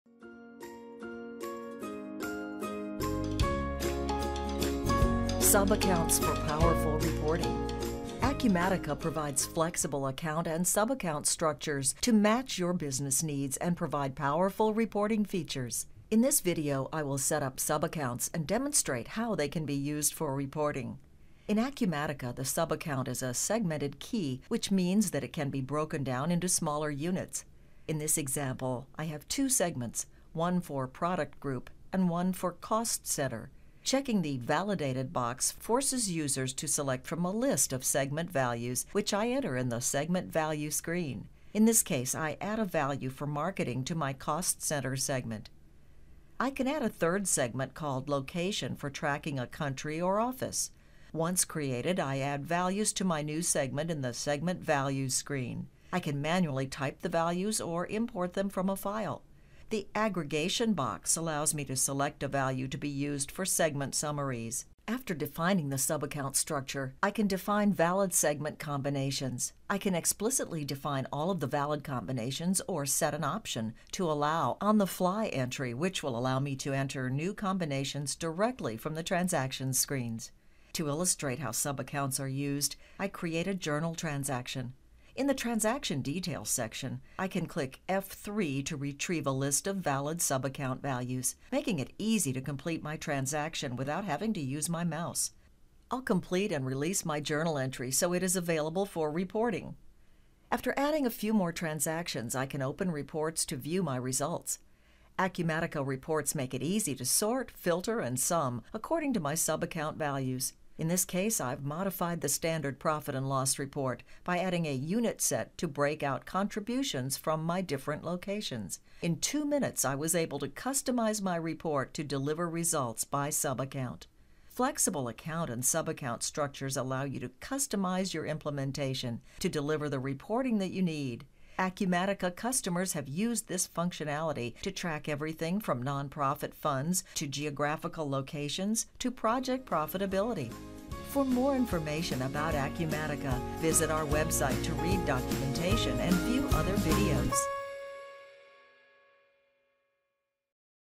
Advanced Reporting using GL Subaccounts in Acumatica
Acumatica provides flexible account and subaccount structures to match your business needs and provide powerful reporting features. This video shows how to create segments and use them for reporting purposes.